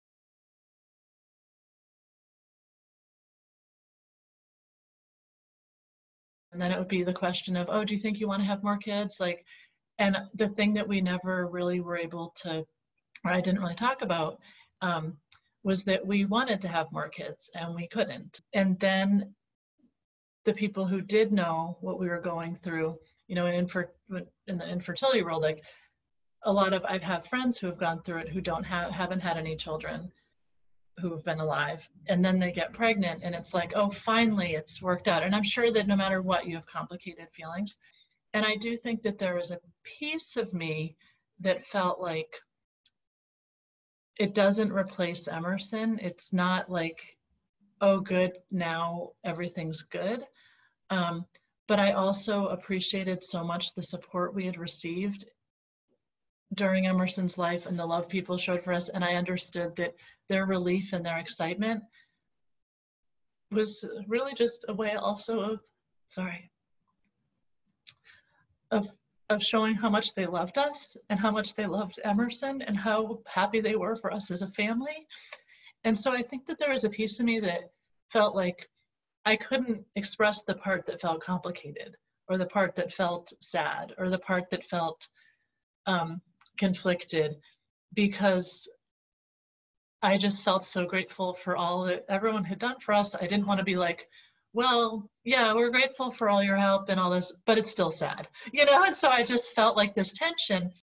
Do you want to have more kids?
The mother of two daughters, the eldest of whom passed away from Type II Gaucher’s Disease at 16 months, talks about the emotionally complicated process of getting pregnant again and feeling like she couldn’t fully express her conflicted emotions to others.